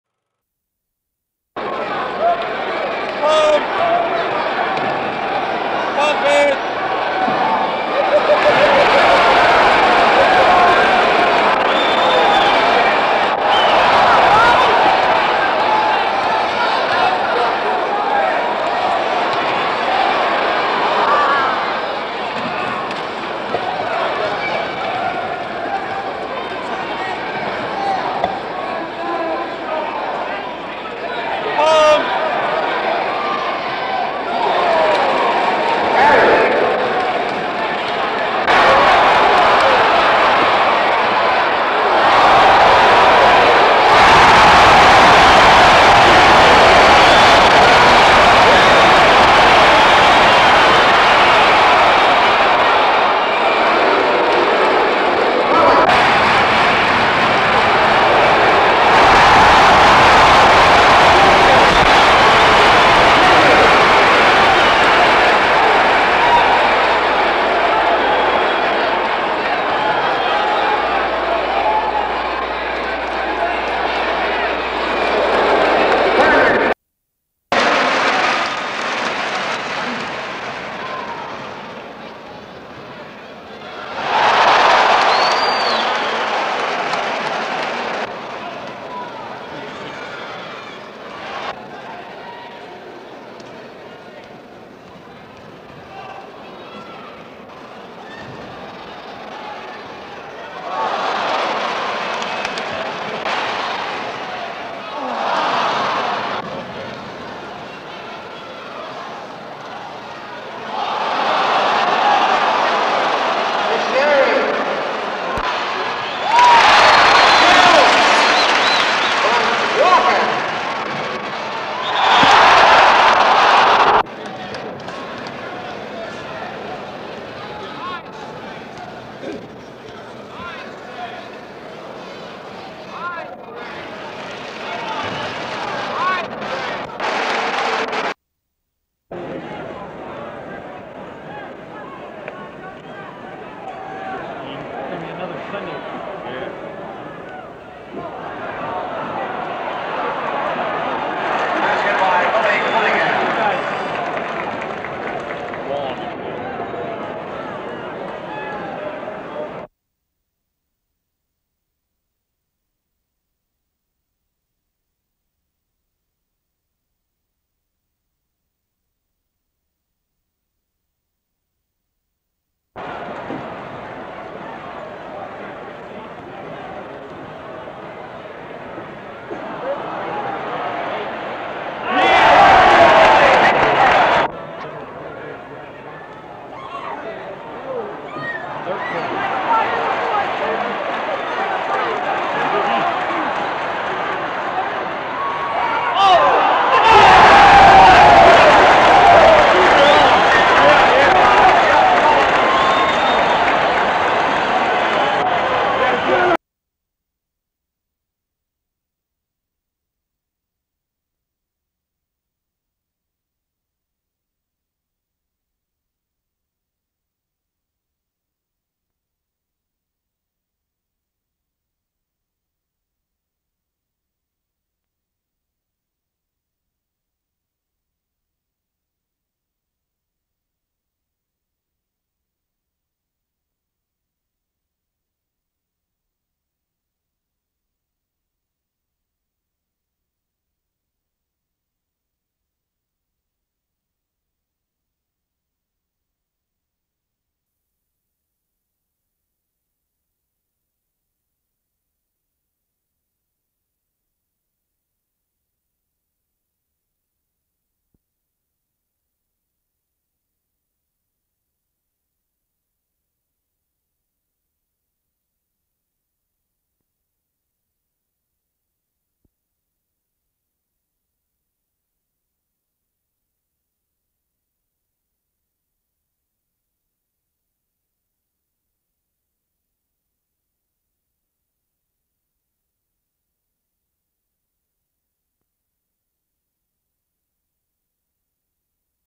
1966-67 NBA Finals San Francisco Warriors vs Philly 76ers Footage Compilation
KPIX-TV News footage compilation shot between April 14-24th 1967 featuring the NBA World Championship Series. This best-of-seven series was played between Western Conference champions the San Francisco Warriors (renamed the Golden State Warriors in 1971) and Eastern Conference champions the Philadelphia 76ers. The 76ers eventually defeated the Warriors 4-2. Includes sound and silent views of on court basketball plays.

Shows that we are not allowed to show here are available there.